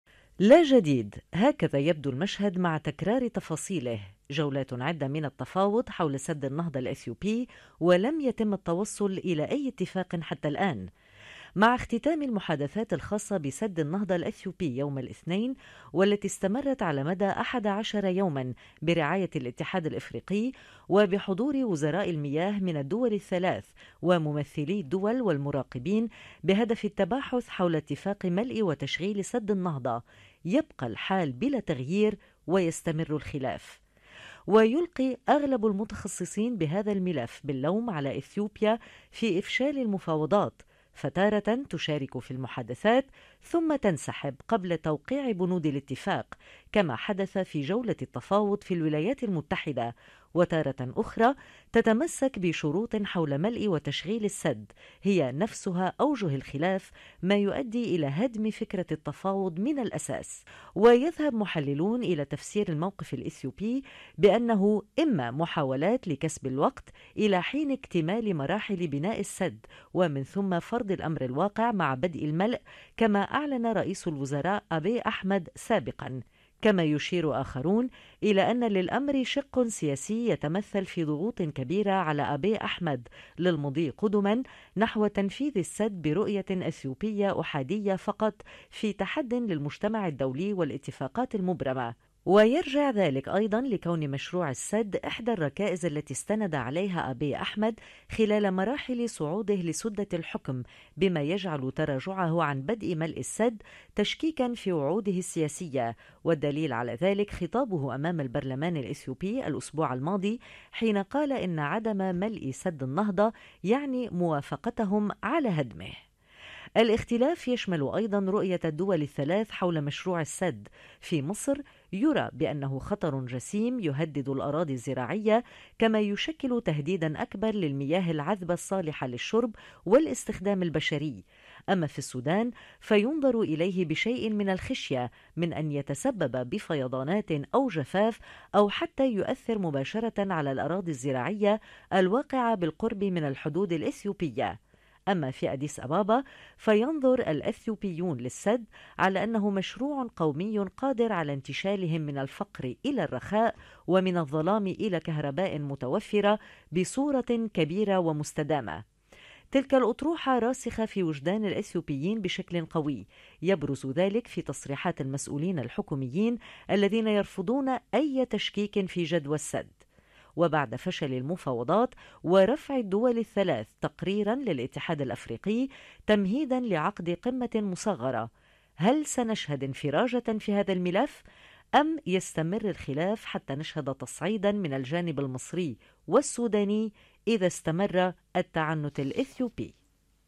لماذا تفشل كل جولات التفاوض حول سد النهضة الإثيوبي؟
أخبار الآن| دبي - الإمارات العربية المتحدة - (أحمد أبو القاسم)

عقب ختام المفاوضات حول سد النهضة والإعلان عن فشل تلك الجولة من المحادثات بين مصر والسودان وإثيوبيا، قررت الدول الثلاثة إرسال تقرير نهائي للاتحاد الإفريقي من أجل عقد قمة مصغرة، هذا الأمر المتكرر يزيد فرضية أن المفاوضات لم تأتي بحل لهذا الملف الشائك، ويبقى السؤال، لماذا تفشل جولات التفاوض حول سد النهضة حتى الآن؟

لا جديد، هكذا يبدو المشهد مع تكرار تفاصيله، جولات عدة من التفاوض حول سد النهضة الإثيوبي، ولم يتم التوصل لأي اتفاق حتى الآن.

مع اختتام المحادثات الخاصة بسد النهضة الأثيوبي، يوم الإثنين والتي إستمرت على مدى 11 يوما برعاية الإتحاد الإفريقي وبحضور وزراء المياه من الدول الثلاث وممثلي الدول والمراقبين بهدف التباحث حول إتفاق ملء وتشغيل سد النهضة، يبقى الحال بلا تغيير ويستمر الخلاف.

ويلقي أغلب المتخصصين بهذا الملف باللوم على إثيوبيا في إفشال المفاوضات، فتارة تشارك في المحادثات, ثم تنسحب قبل توقيع بنود الاتفاق كما حدث في جولة التفاوض في الولايات المتحدة، وتارة أخرى تتمسك بشروط حول ملء وتشغيل السد، هي نفسها أوجه الخلاف ما يؤدي الى هدم فكرة التفاوض من الأساس.

ويذهب محللون لتفسير الموقف الإثيوبي بأنه إما محاولات لكسب الوقت الى حين إكتمال مراحل بناء السد , ومن ثم فرض الأمر الواقع مع بدء الملء,  كما أعلن رئيس الوزراء آبي أحمد سابقا، كما يشير آخرون بأن للأمر شق سياسي يتمثل في ضغوط كبيرة على آبي أحمد للمضي قدما نحو تنفيذ السد برؤية إثيوبية أحادية فقط في تحد للمجتمع الدولي والاتفاقات المبرمة. ويرجع ذلك ايضا لكون مشروع السد إحدى الركائز التي استند عليها آبي أحمد خلال مراحل صعوده لسدة الحكم، بما يجعل تراجعه عن بدء ملء السد تشكيكا في وعوده السياسية، والدليل على ذلك، خطابه أمام البرلمان الإثيوبي الاسبوع الماضي حين قال إن عدم ملء سد النهضة يعني موافقتهم على هدمه.

الاختلاف يشمل ايضا رؤية الدول الثلاث حول مشروع السد، في مصر يُرى بأنه خطر جسيم يهدد الأراضي الزراعية، كما يشكل تهديدا أكبر للمياه العذبة الصالحة للشرب والاستخدام البشري.

أما في السودان، فينظر إليه بشىء من الخشية من أن يتسبب بفيضانات أو جفاف أو حتى يؤثر مباشرة على الأراضي الزراعية الواقعة بالقرب من الحدود الإثيوبية.

أما في أديس أبابا، فينظر الإثيوبيون للسد على انه مشروع قومي قادر على إنتشالهم من الفقر إلى الرخاء، ومن الظلام إلى كهرباء متوفرة بصورة كبيرة ومستدامة. تلك الأطروحة راسخة في وجدان الإثيوبيين بشكل قوي، يبرز ذلك في تصريحات المسؤولين الحكوميين الذين يرفضون أي تشكيك في جدوى السد .

وبعد فشل المفاوضات، ورفع الدول الثلاث تقريرا للاتحاد الإفريقي تمهيدا لعقد قمة مصغرة، هل سنشهد إنفراجة في هذا الملف، أما يستمر الخلاف حتى نشهد تصعيدا من الجانب المصري والسوداني إذا استمر التعنت الإثيوبي.